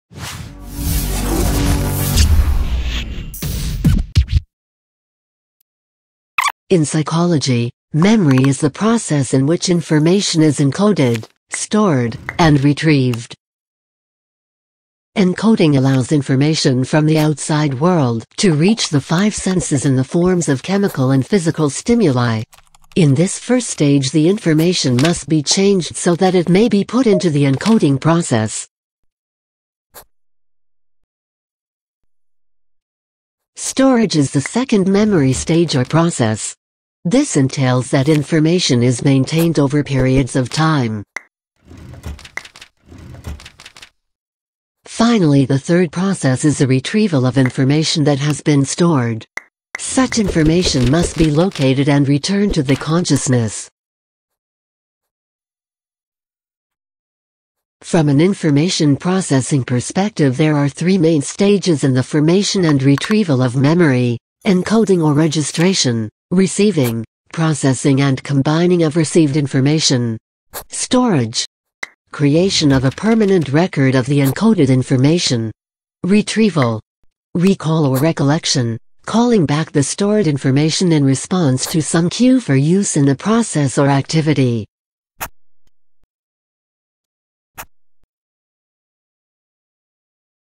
MEMORY
In psychology, memory is the process in which information is encoded, stored, and retrieved. Encoding allows information from the outside world to reach the five senses in the forms of chemical and physical stimuli. In this first stage the information must be changed so that it may be put into the encoding process. Storage is the second memory stage or process. This entails that information is maintained over periods of time. Finally the third process is the retrieval of information that has been stored. Such information must be located and returned to the consciousness. Some retrieval attempts may be effortless due to the type of information, and other attempts to remember stored information may be more demanding for various reasons.

From an information processing perspective there are three main stages in the formation and retrieval of memory:

    Encoding or registration: receiving, processing and combining of received information
    Storage: creation of a permanent record of the encoded information
    Retrieval, recall or recollection: calling back the stored information in response to some cue for use in a process or activity